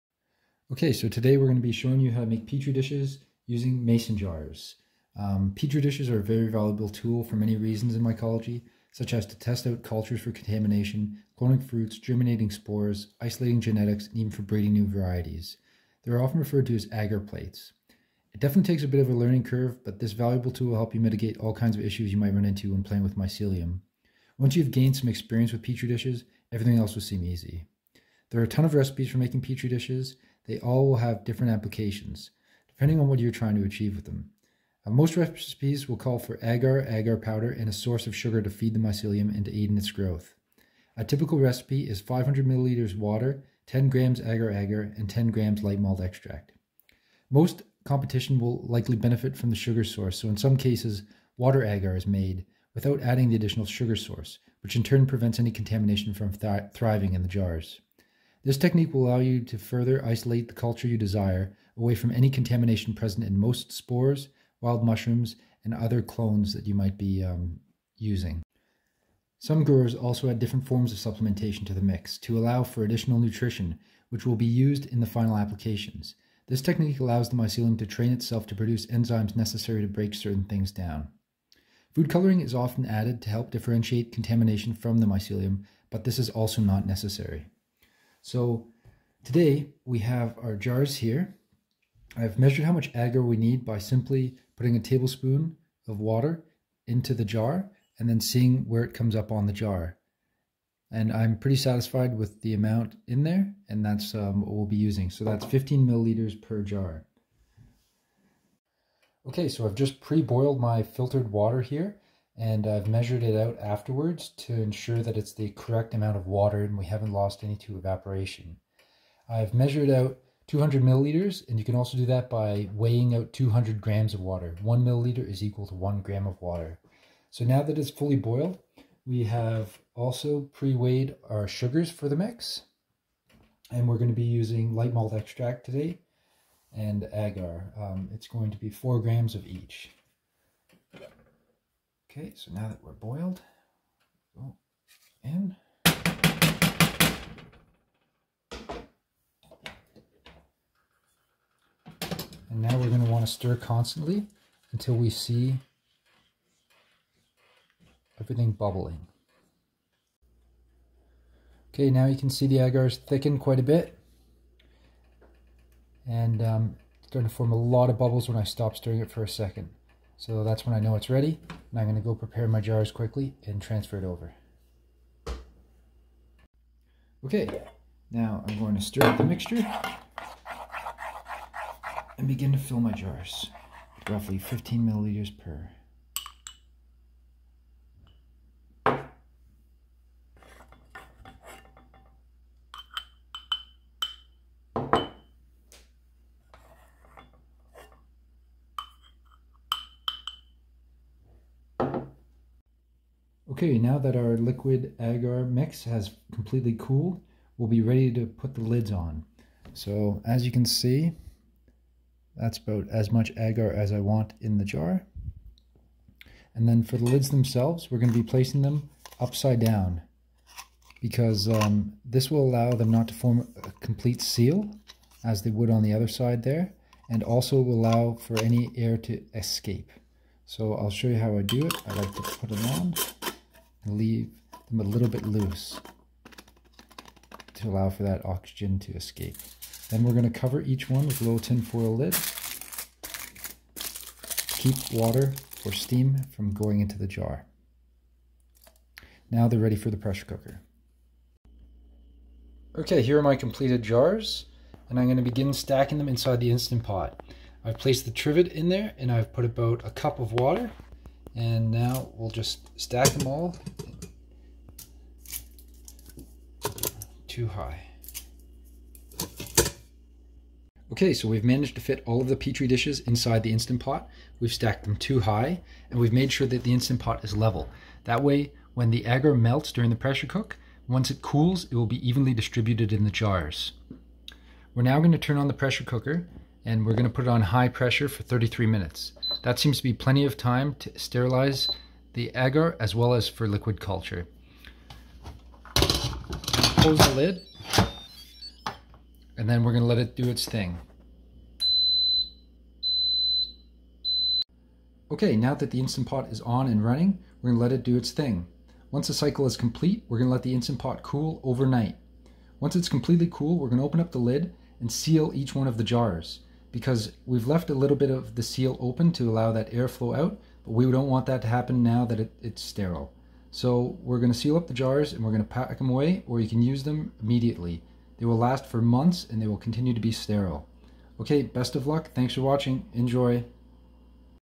No-pour agar jars using an Instant Pot (petri dishes)
In this video I will show you how to make sterile petri dishes for mycology using mason jars and pressure cooking them, using the instant pot. You can reuse these mason jars over and over, eliminating the need for single use plastic petri dishes.

By using this technique of pressure cooking the agar in the jars, you are able to avoid contamination that may occur when pouring agar in a non-sterile environment/without a flow hood. This method is called no pour, because you pour the agar into the jars while it is not sterile, prior to pressure cooking it.

Pressure cook jars in the instant pot on high pressure for 33 minutes, let cool for 12 hours or overnight and remove jars, and tighten the lids.

There are hundreds of recipes you can use for Petri dishes, here are some good ones for beginners to start with.

Water agar  500ml recipe
  (by not adding sugar = less chance of contamination)
-500ml filtered water/non-chlorinated
-10 grams agar powder

-optional food colouring (1-2 drops)
Water

Malt extract agar  500ml recipe

-500ml filtered water/non-chlorinated
-10 grams agar powder

-10 grams light malt extract

Malt Yeast extract agar  500ml recipe

-500ml filtered water/non-chlorinated

-10 grams agar powder

-10 grams light malt extract
-1 gram nutritional yeast (grind it up into a powder first)

MYEA

*You can also add several other specific things into the agar to help train the mycelium to produce enzymes and get familiar with breaking down future substrates.
i.e. sawdust, bran, grain flower, gypsum, coconut coir, soy hulls, activated charcoal etc.